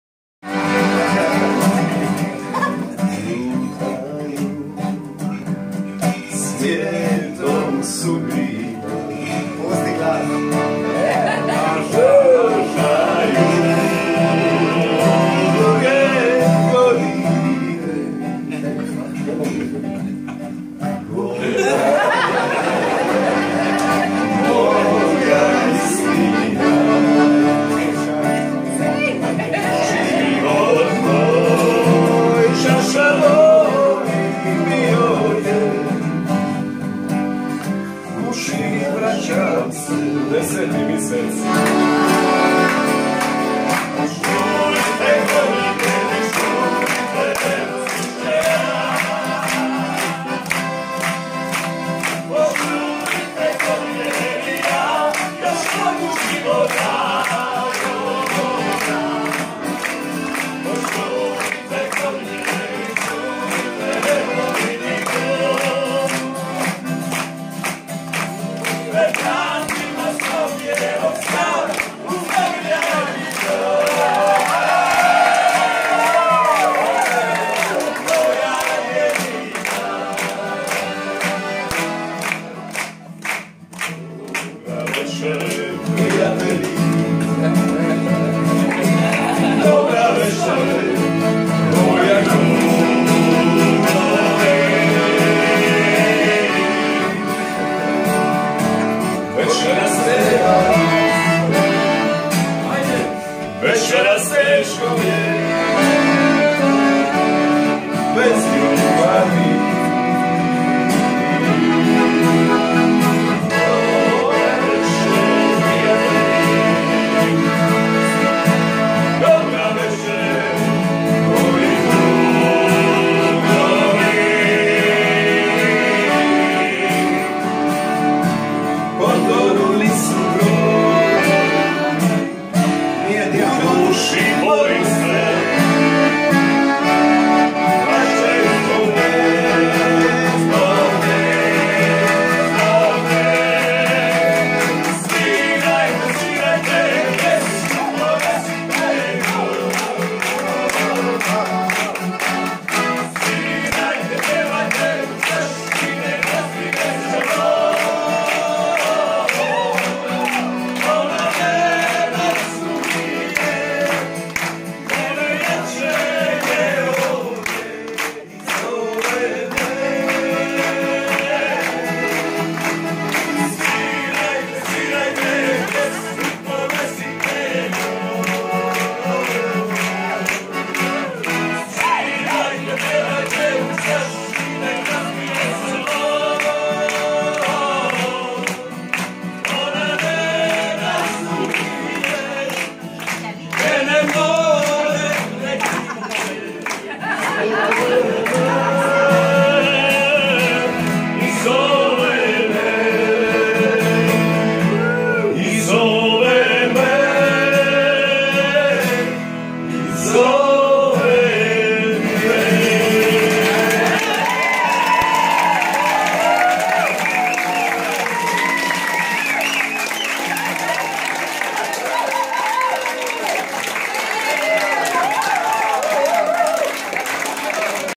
La cena del capitano sulla Diamond - parte prima
Venerdì sera 6 maggio 2017 a Makaraska, una delle tante isole della Croazia. Un membro dell'equipaggio, Angelo, si esibisce come cantante.